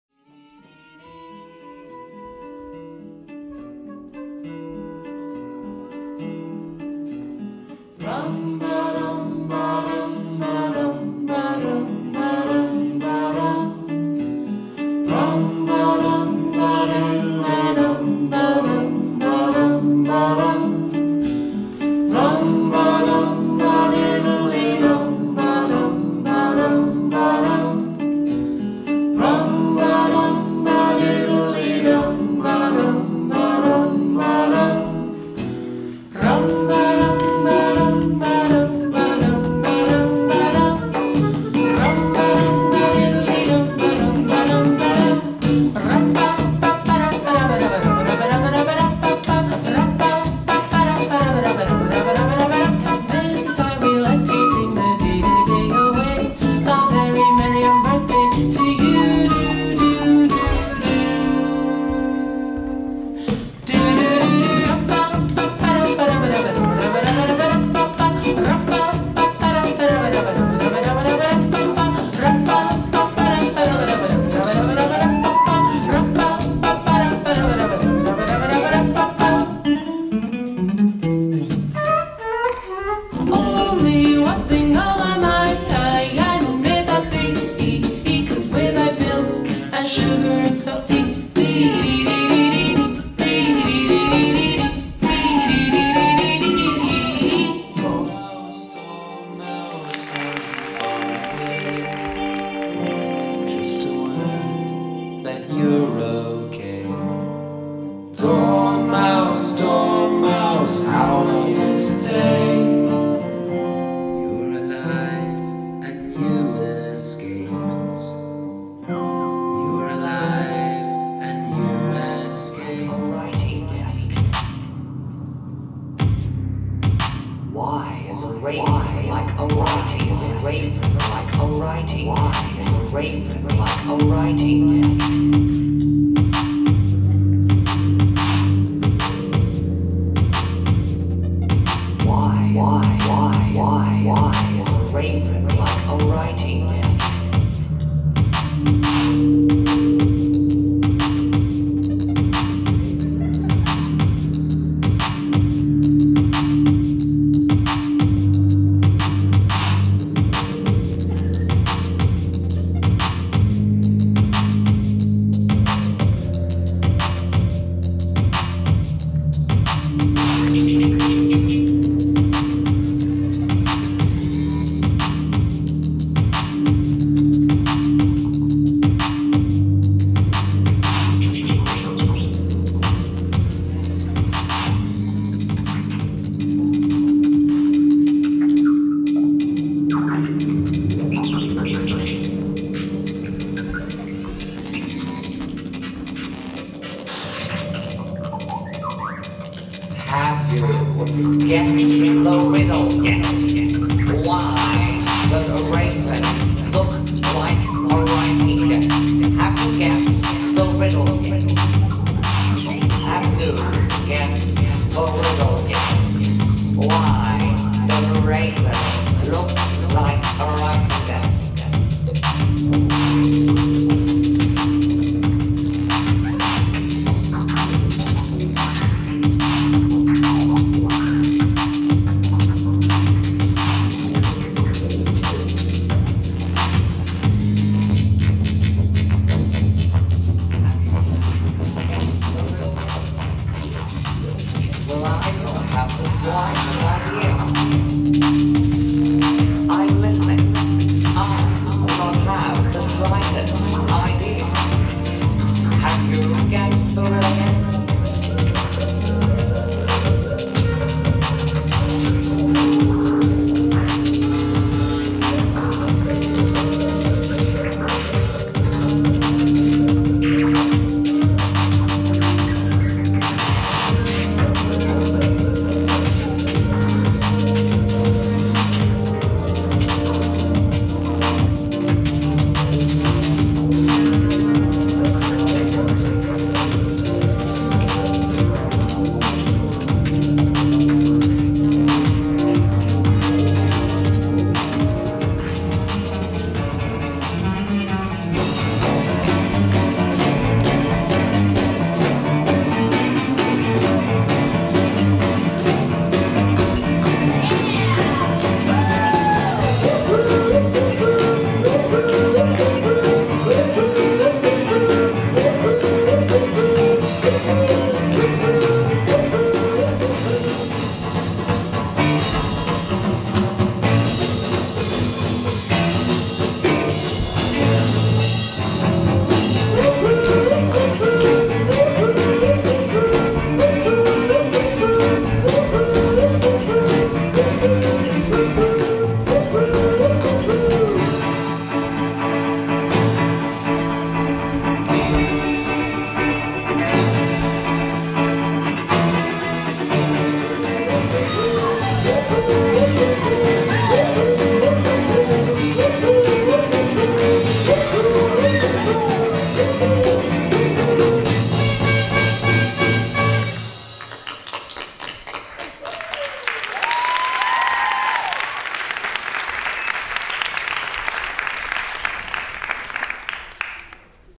COLLABORATION THE PROJECT - The Tea Party
Excerpt from Paul Malek's Down The Rabbit Hole
Performed By Stephen Agisilaou Kim Adam Bree Langridge & Nick Geurts